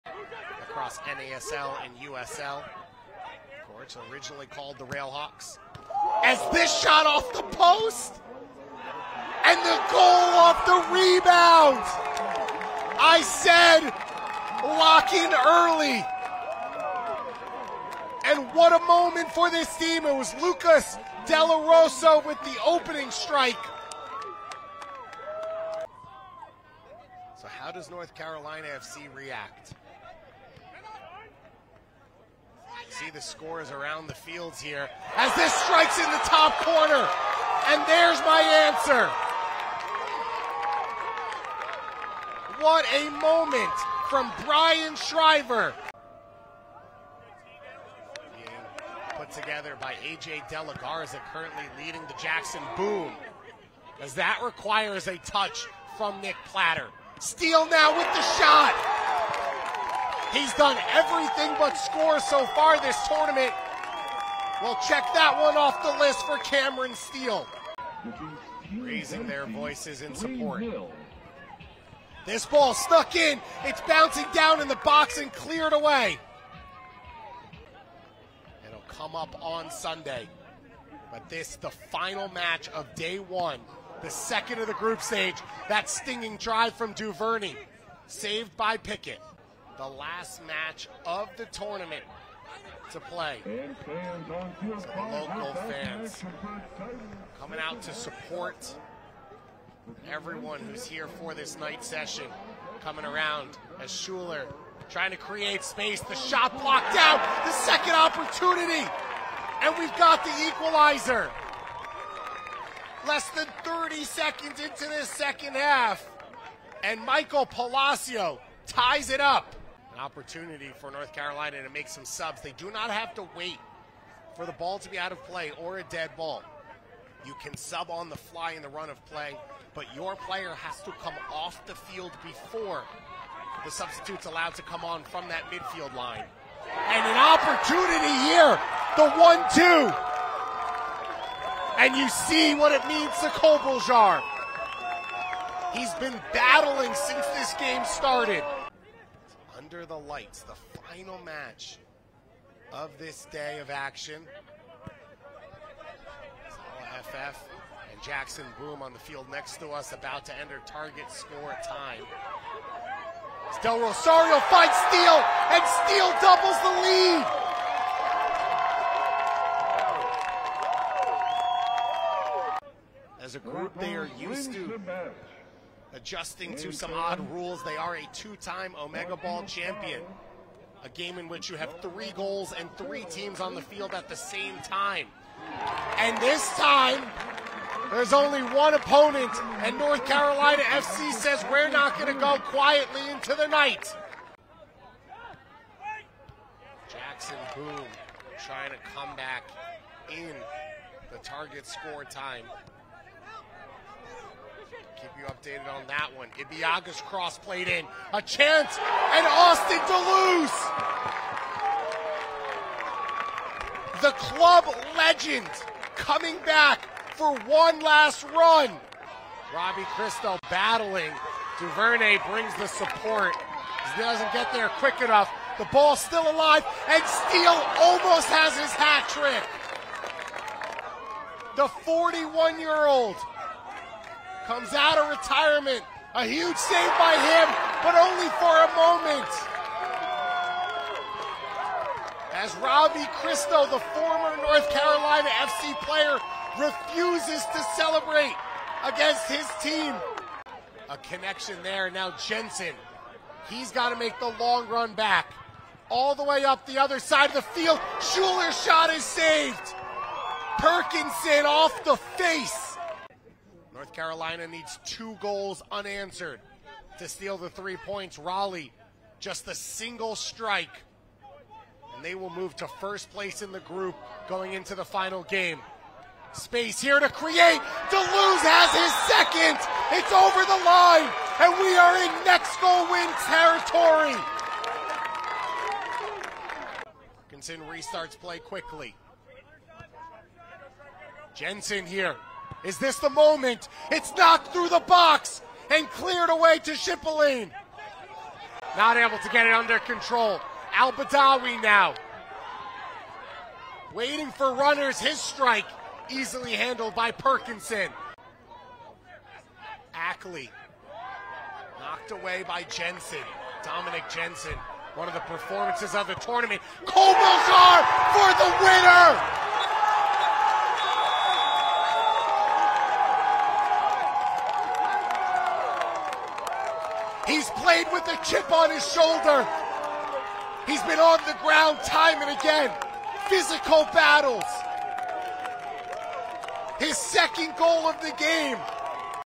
TST 2023 REWIND: Raleigh Rebels vs NCFC
Final score: Raleigh Rebels 6 - NCFC 5

TST is the World Championship of 7v7 Soccer. 48 team men's tournament. 8 team women's tournament. Two $1 million champions.

Follow TST on social